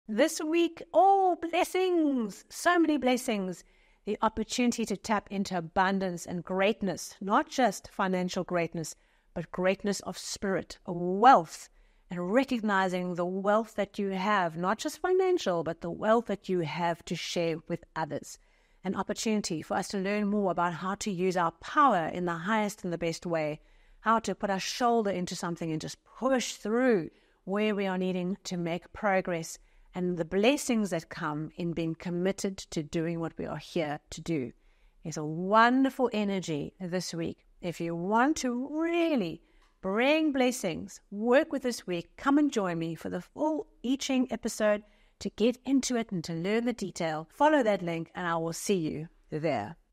I Ching Weekly To Go 4-10 Mar 2024
Weekly I Ching and summary of what to focus on this coming week.

WOW! A profound opportunity awaits us this week, says the I Ching, with wealth and prosperity abounding on whatever level we are working with it. Plus we have great power to push forward and achieve something wonderful.